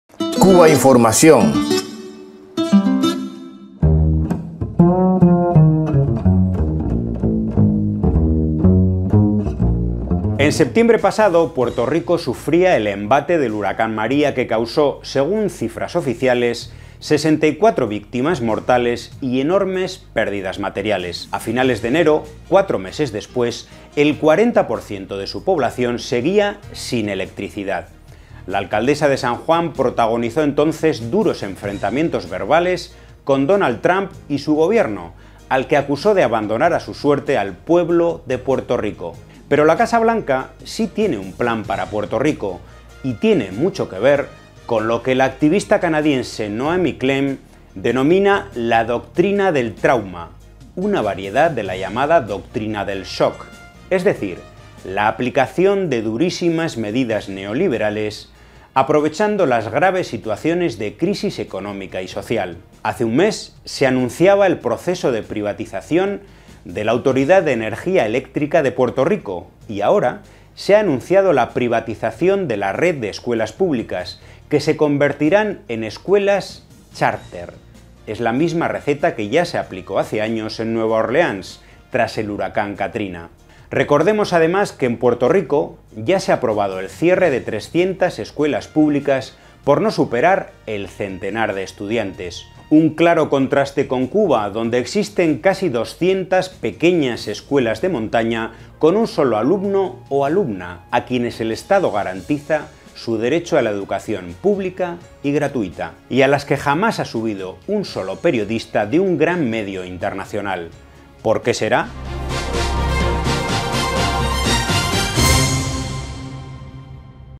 Puerto Rico: doctrina del trauma y privatización de escuelas
Cubainformación TV – Basado en un texto de Ivana Belén Ruiz.- En septiembre pasado, Puerto Rico sufría el embate del huracán María, que causó –según cifras oficiales- 64 víctimas y enormes pérdidas materiales. A finales de enero, cuatro meses después, el 40 % de su población seguía sin electricidad. Edición: Ana Gil. Fuente